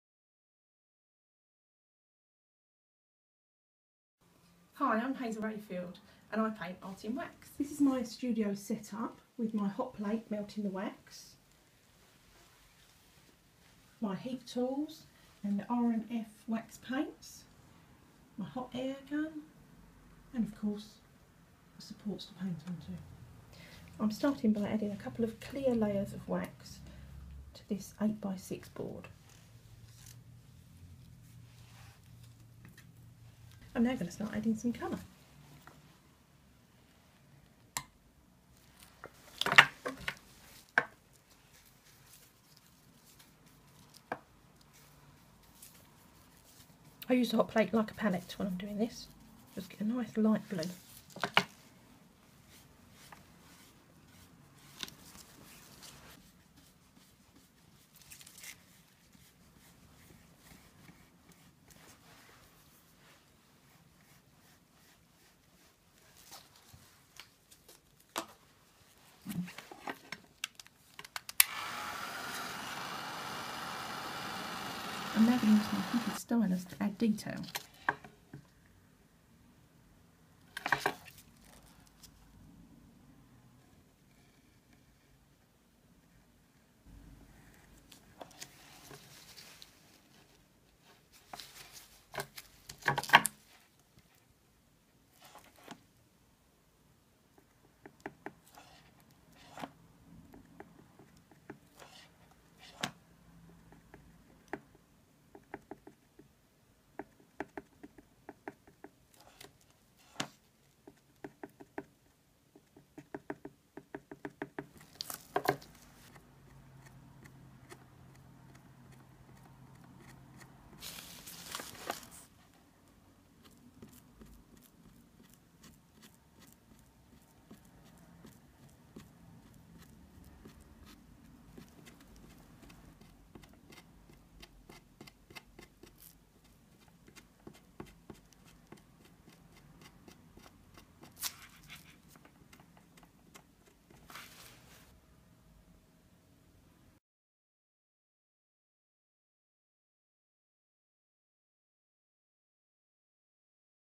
Encaustic Painting Lavender Art In Wax
Hazel Rayfield shows you around her Art In Wax Studio set up and then demonstrates painting a Lavender picture in hot wax, using an Ampersand Arts Encausticbord and R & F Paints handmade wax paints. Read more on the Art In wax blog